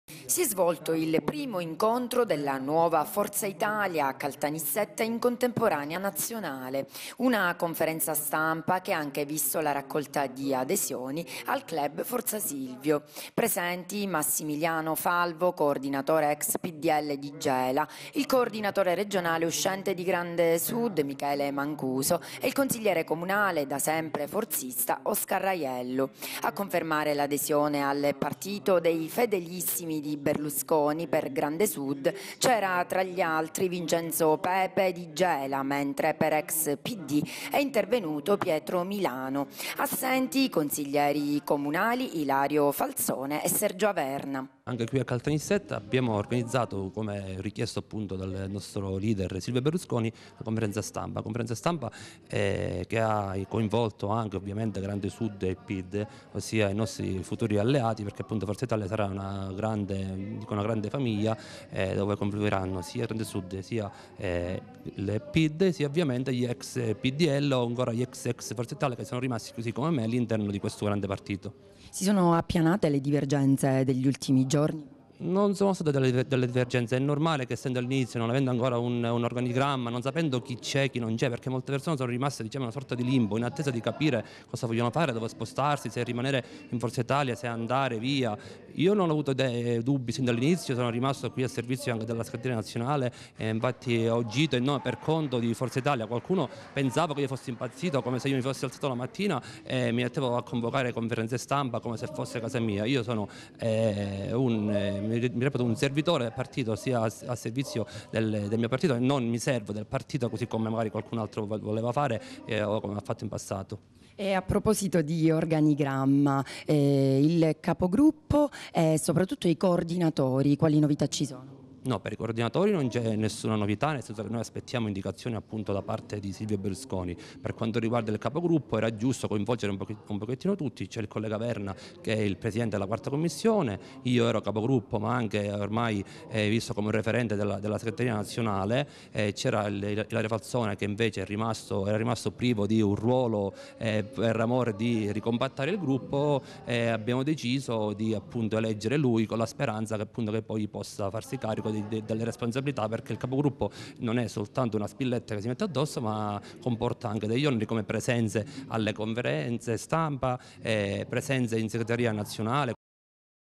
FORZA ITALIA LA PRESENTAZIONE
CONFERENZA STAMPA DI PRESENTAZIONE DELLA NUOVA FORZA ITALIA IN PROVINCIA